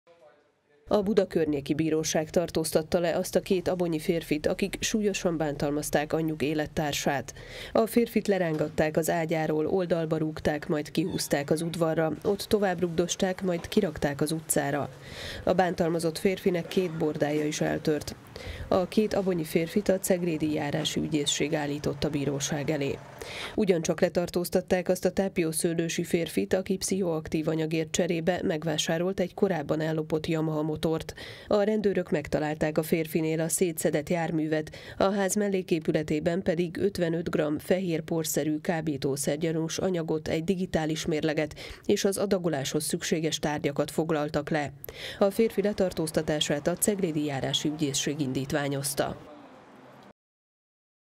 Három férfit tartóztattak le
Két abonyi férfit súlyos testi bántalmazásért, egy tápiószőlősi férfit pedig kábítószerrel való üzletelésért vettek őrizetbe a rendőrök, és már el is rendelték előzetes letartóztatásukat.